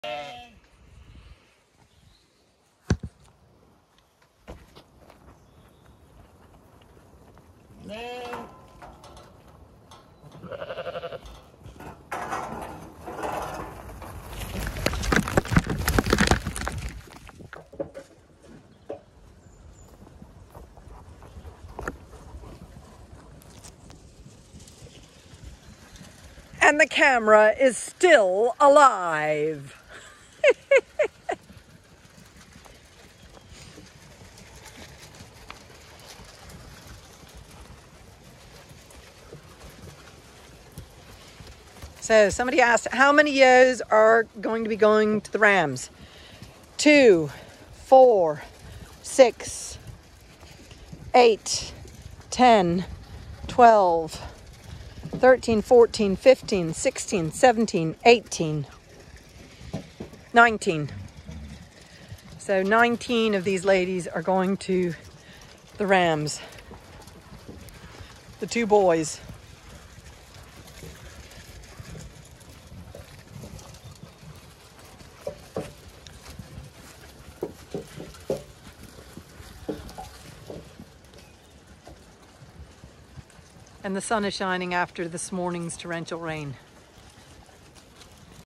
Sun came out to feed ewes & counted ewes for this year’s breeding
It really is beautiful when the sun comes out after all this morning’s rain.